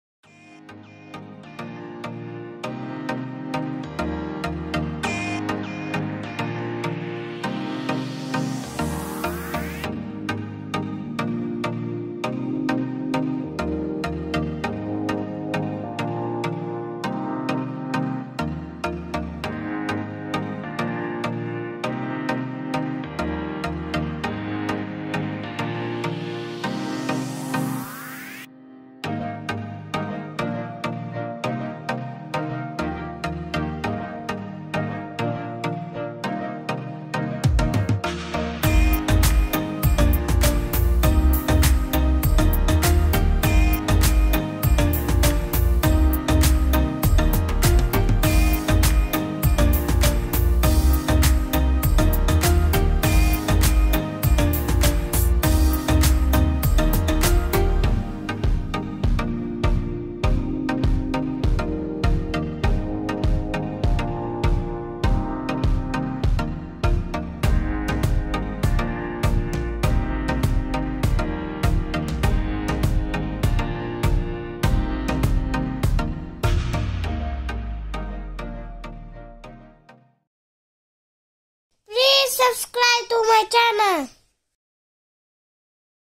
How to Draw a colourful Race Car | Sport Car | Formula 1 car for kids, easiest drawing
In this video, we will be creating a step-by-step easy drawing for kids of Sports Car and coloring it. Follow my Steps Sport Car drawing and you will be able to draw as beautiful car as mine. This is so easy Sport Car drawing for kids and for beginners.

Your support really encourages us to continue creating easy drawing tutorials every day.